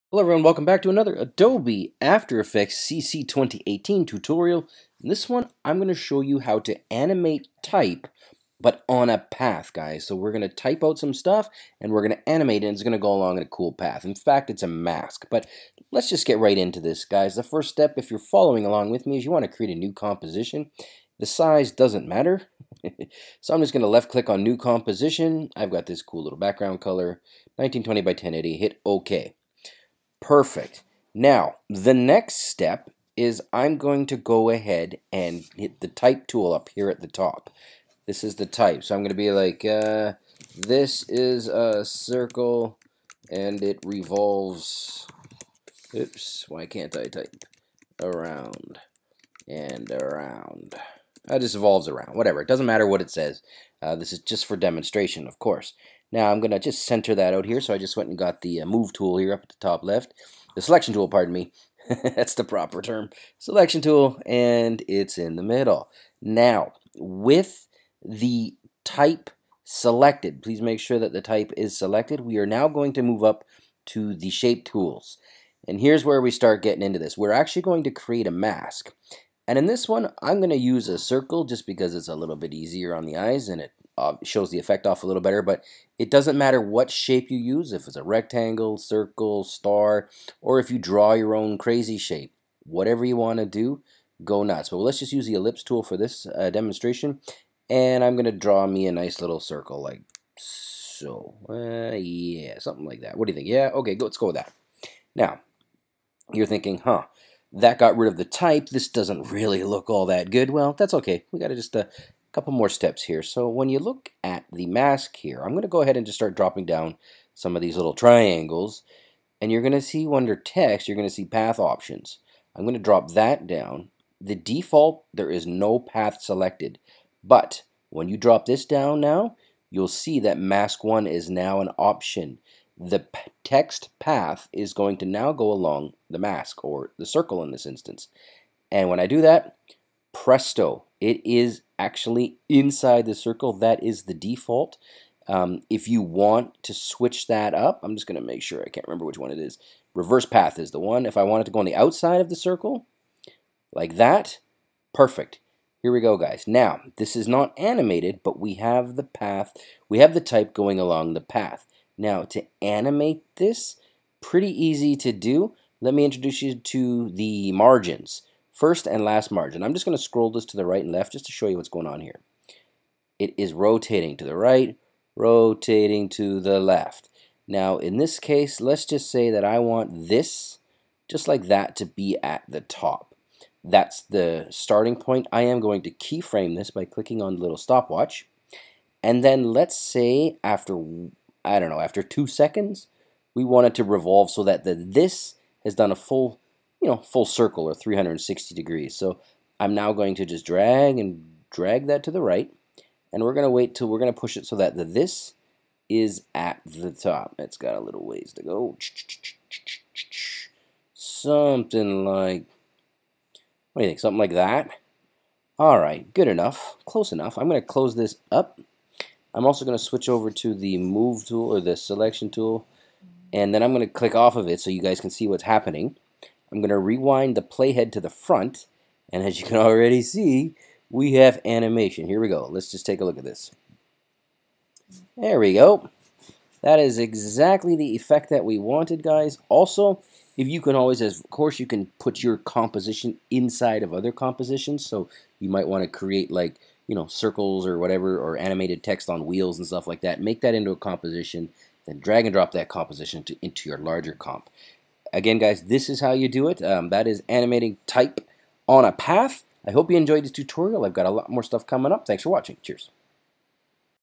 How to Animate Type on a path - Adobe After Effects
In this video, I show how to to animate type on a path inside adobe after effects cc 2018.  First step is to create a new composition, then click on the type tool.  Type out what you want.  Next, making sure you are clicked on the type layer, click on the shape tool.  Draw out a shape / mask.  Then click on the text layer, drop down to path options,, and then make sure the path is set to Mask 1.  For the animation, simply keyframe the first margin or last margin.